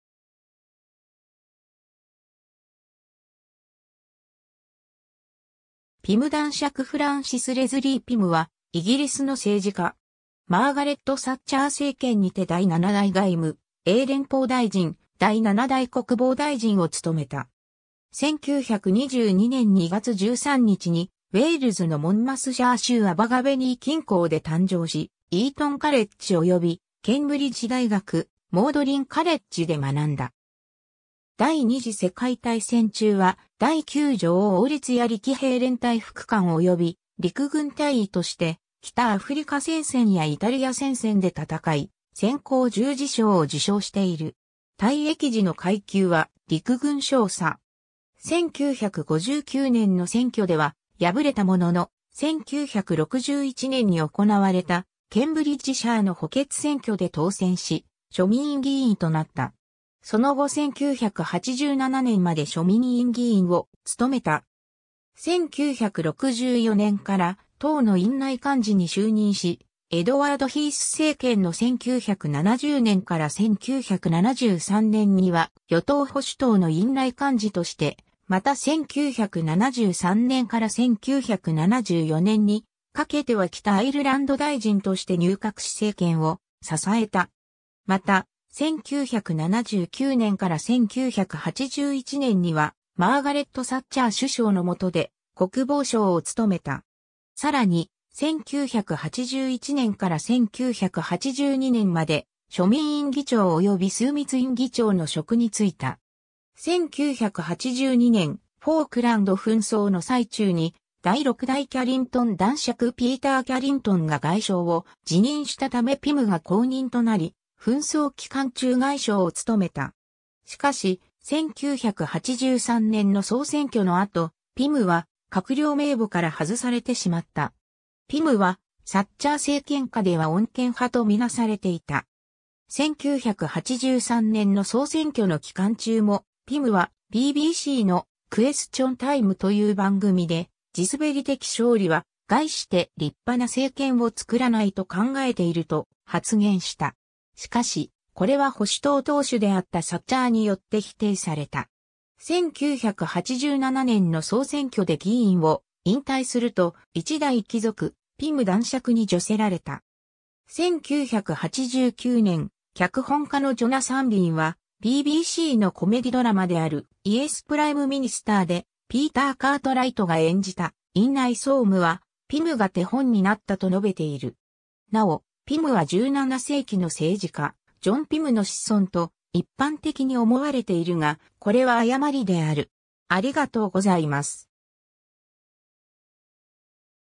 フランシス・ピム
ピム男爵フランシス・レズリー・ピム（英語: Francis Leslie Pym, Baron Pym, MC, PC, 1922年2月13日 - 2008年3月7日）は、イギリスの政治家。
マーガレット・サッチャー政権にて第7代外務・英連邦大臣、第7代国防大臣を務めた。
 1922年2月13日にウェールズのモンマスシャー州アバガヴェニー近郊で誕生し、イートン・カレッジ及びケンブリッジ大学モードリン・カレッジで学んだ。
第二次世界大戦中は、第9女王王立槍騎兵連隊副官及び陸軍大尉として北アフリカ戦線やイタリア戦線で戦い、戦功十字章を受章している。
退役時の階級は陸軍少佐。
 1959年の選挙では敗れたものの、1961年に行われたケンブリッジシャーの補欠選挙で当選し、庶民院議員となった。
その後1987年まで庶民院議員を務めた。
 1964年から党の院内幹事に就任し、エドワード・ヒース政権の1970年から1973年には与党保守党の院内幹事として、また1973年から1974年にかけては北アイルランド大臣として入閣し政権を支えた。
また、1979年から1981年には、マーガレット・サッチャー首相の下で国防相を務めた。
さらに、1981年から1982年まで庶民院議長および枢密院議長の職に就いた。
1982年、フォークランド紛争の最中に第6代キャリントン男爵ピーター・キャリントンが外相を辞任したためピムが後任となり、紛争期間中外相を務めた。
しかし、1983年の総選挙の後、ピムは閣僚名簿から外されてしまった。
 ピムは、サッチャー政権下では穏健派（Wets）と見なされていた。
1983年の総選挙の期間中も、ピムはBBCの「クエスチョン・タイム」という番組で「地すべり的勝利は、概して立派な政権を作らない」と考えていると発言した。
しかし、これは保守党党首であったサッチャーによって否定された。
 1987年の総選挙で議員を引退すると、一代貴族ピム男爵に叙せられた。
1989年、脚本家のジョナサン・リンは、BBCのコメディドラマである「イエス・プライム・ミニスター」でピーター・カートライトが演じた院内総務は、ピムが手本になったと述べている。
 なお、ピムは17世紀の政治家ジョン・ピムの子孫と一般的に思われているが、これは誤りである。